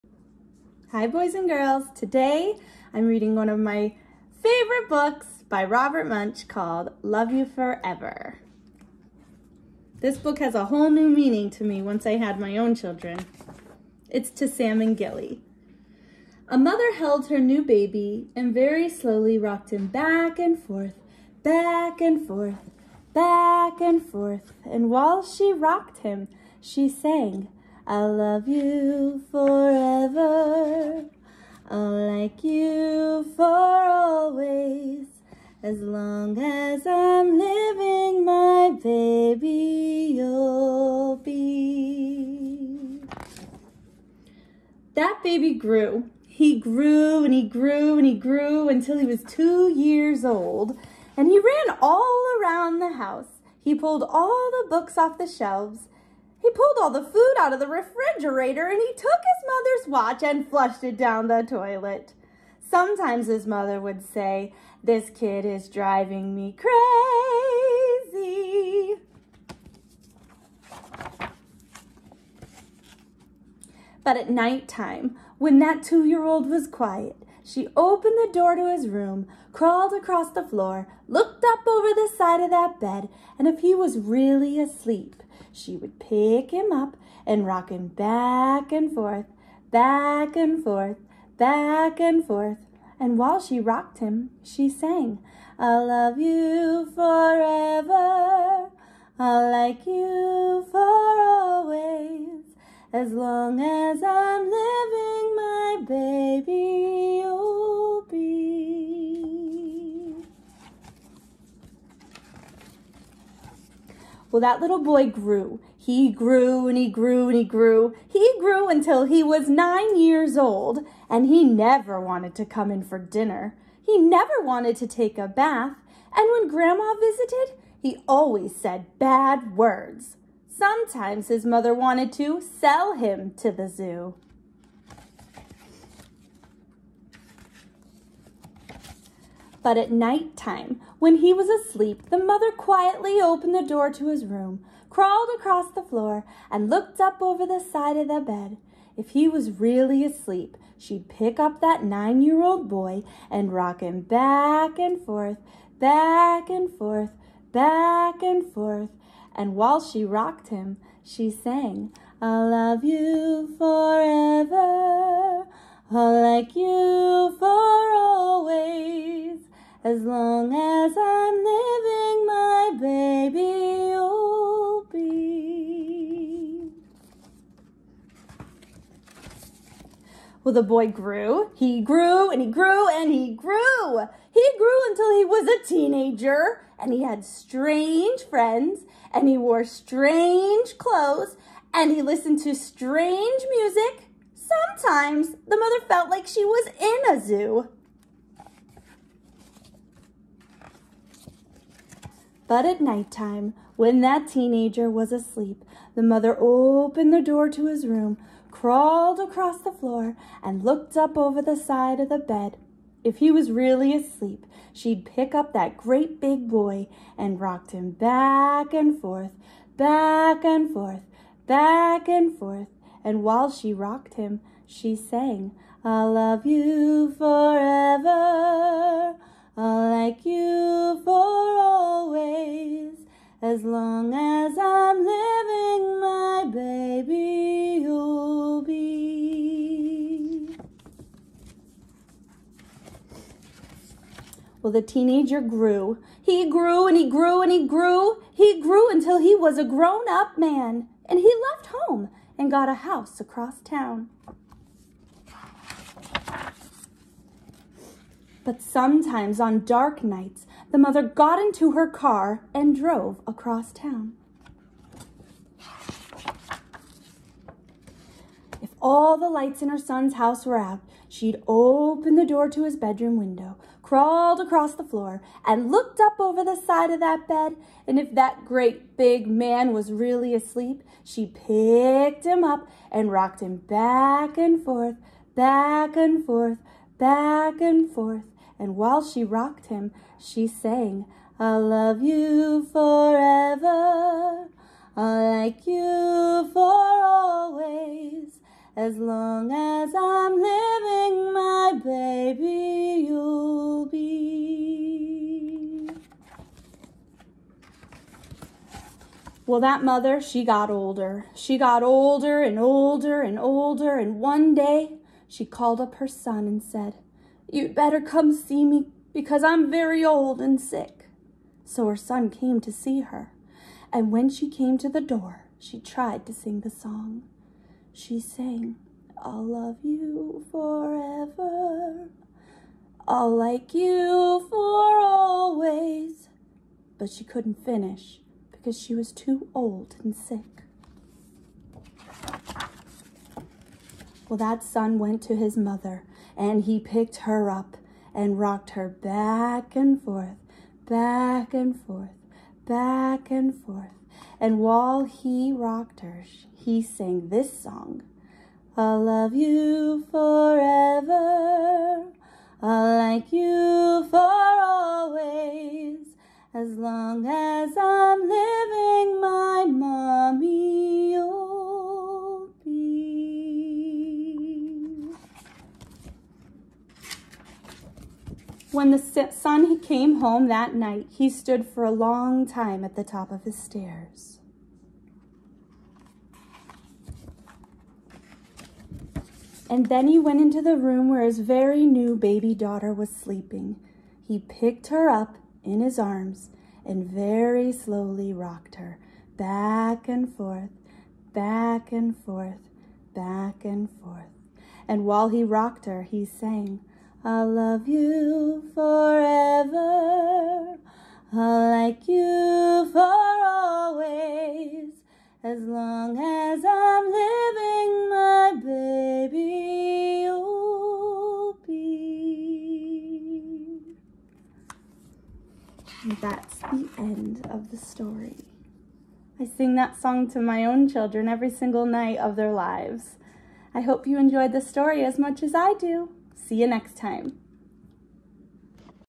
Love You Forever By: Robert Munsch
The love between a mother and son is undeniable. This is such a touching story.